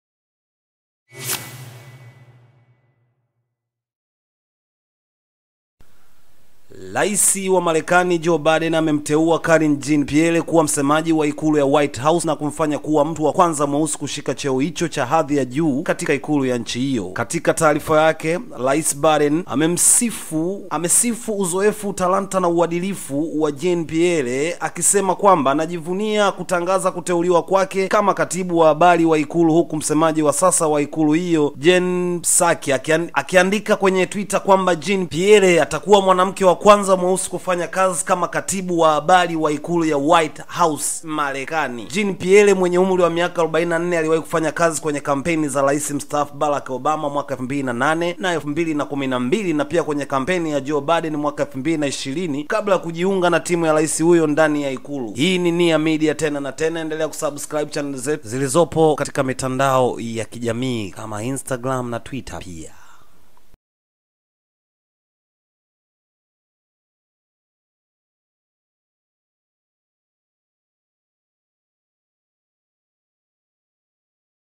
JOE BIDEN AMTEUWA MSEMAJI MWEUSI WA IKURU YA MAREKANI
Rais Wa Marekani Joe Biden Amteuwa Karine Jean-Pierre Kuwa Msemaji Mkuu Wa Ikulu Ya Marekani, Karine Jean Pierre Ni Mwafrika Wa Kwanza Kushika Wadhifa Huo  Katika Nchi Ya Marekani